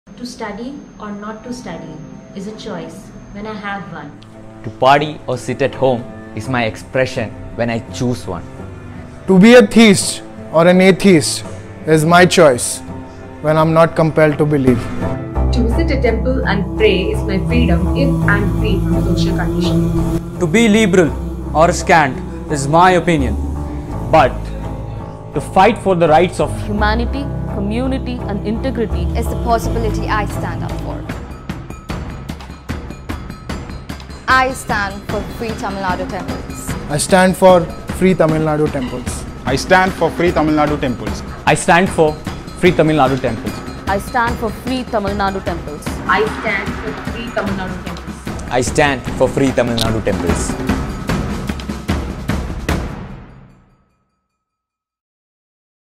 The Youth have spoken!!! Free Tamil Nadu temples 🙏🏾
We the youth of Bharath demand this. Free our temples. Let the devotees manage them🙏🏾